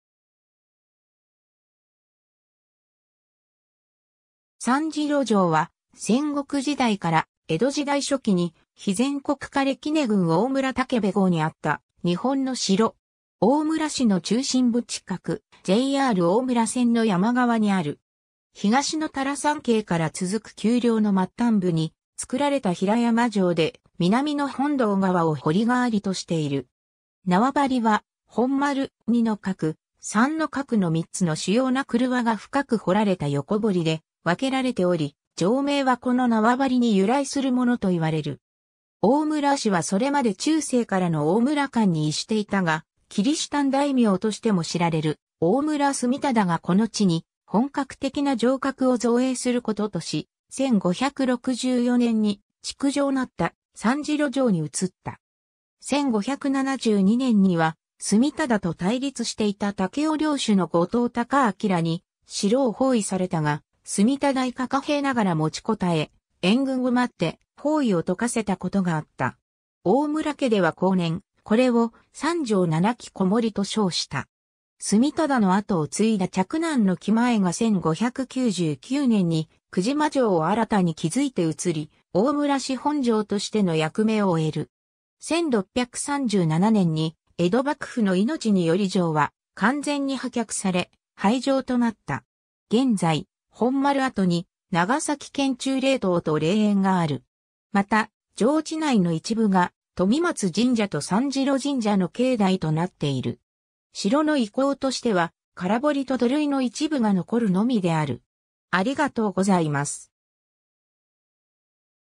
三城城
三城城（さんじょうじょう）は、戦国時代から江戸時代初期に肥前国彼杵郡大村武部郷（現長崎県大村市三城町）にあった日本の城。
 大村市の中心部近く、JR大村線の山側にある。
東の多良山系から続く丘陵の末端部に造られた平山城で、南の本堂川を堀代わりとしている。
 縄張りは本丸、二の郭、三の郭の三つの主要な曲輪が深く掘られた横堀（空堀）で分けられており、城名はこの縄張りに由来するものといわれる。
 大村氏はそれまで中世からの大村館（城の北西、乾馬場町付近）に居していたが、キリシタン大名としても知られる大村純忠がこの地に本格的な城郭を造営することとし、1564年（永禄7年）に築城成った三城城に移った。
1572年（元亀3年）には純忠と対立していた武雄領主の後藤貴明らに城を包囲されたが純忠以下寡兵ながら持ちこたえ、援軍を待って包囲を解かせたことがあった。
大村家では後年、これを「三城七騎篭り」と称した。
 純忠の跡を継いだ嫡男の喜前が1599年（慶長4年）に玖島城を新たに築いて移り、大村氏本城としての役目を終える。
1637年（寛永14年）に江戸幕府の命により城は完全に破却され廃城となった。
 現在、本丸跡に長崎県忠霊塔（1934年建立）と霊苑がある。
また、城地内の一部が富松神社と三城神社の境内となっている。
城の遺構としては空堀と土塁の一部が残るのみである。